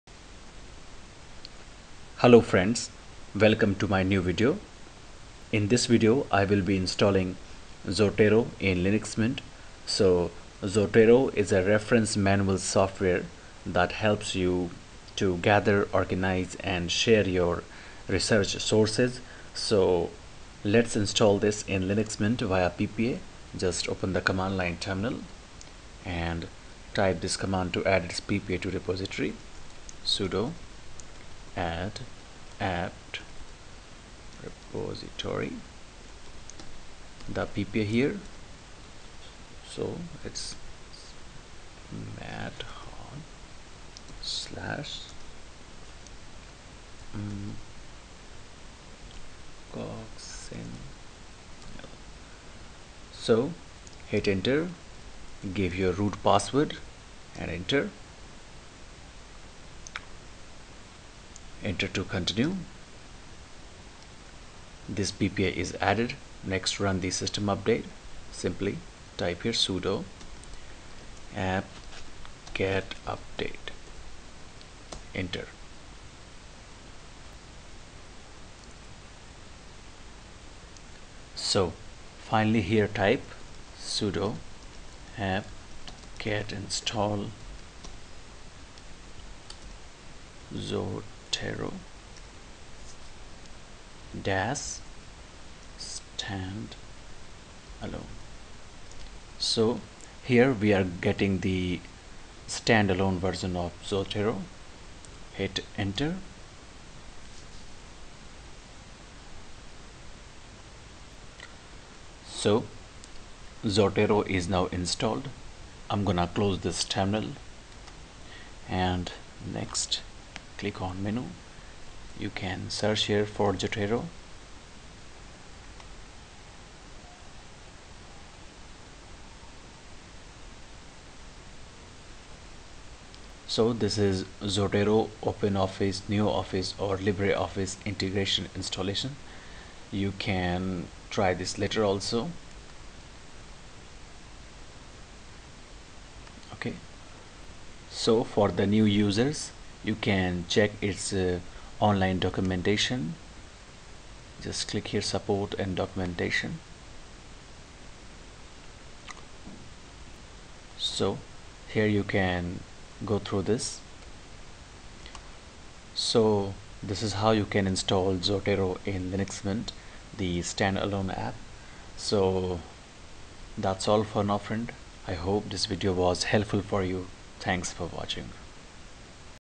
Install Zotero Standalone Version ( Reference Management Software) in Linux Mint Via PPA
Commands for installation
sudo add-apt-repository ppa:smathot/cogscinl
sudo apt-get update
sudo apt-get install zotero-standalone